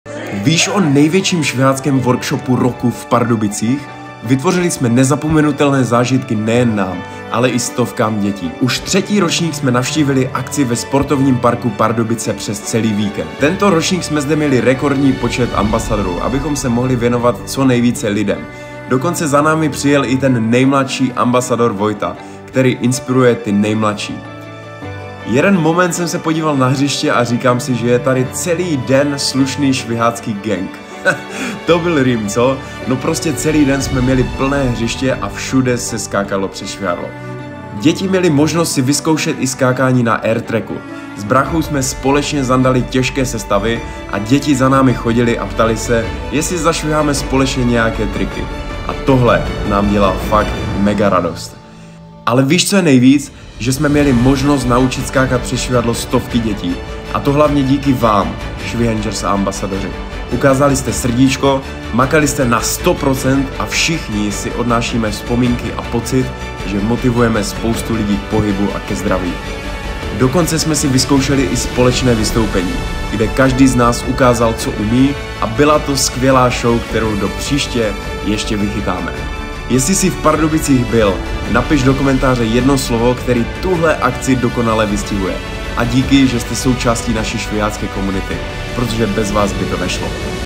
Že jsme byli na workshopu v Pardubicích, už asi víš 🤭.
Že se nám povedl, to už jsi nejspíš taky registroval/a 🏆.

Ale že je to topka mezi našimi workshopy a prostě dokonale VY LA ZE NÁ akce.. prostě tady jsme si ještě připravili minutu a půl srdeční záležitosti 💛💛.

Tak zase příště? ☀️ @sportovni_park_pardubice

Pro více informací o našich workshopech budoucích i minulých, koukni do našich IG výběrů! 🤪